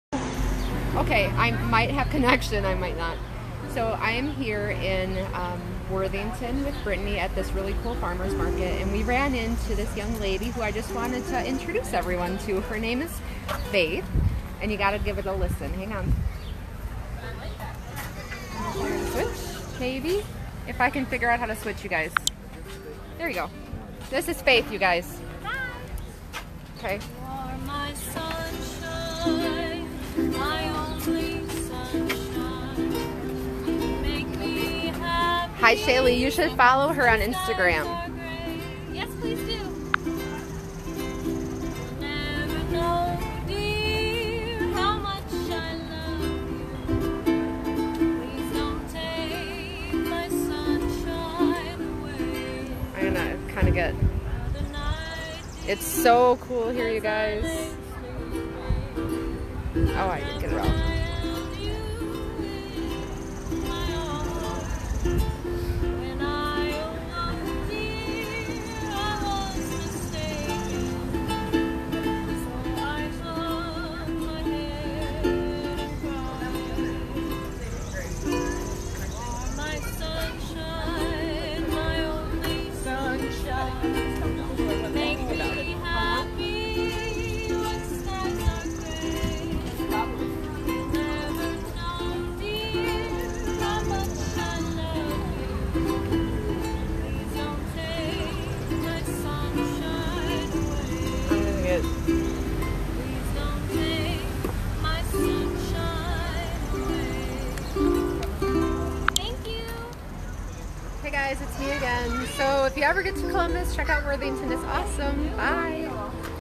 Faith in Ohio
When I was visiting a good friend in Ohio, we went to a couple Farmers Market and had the chance to meet this amazing young lady!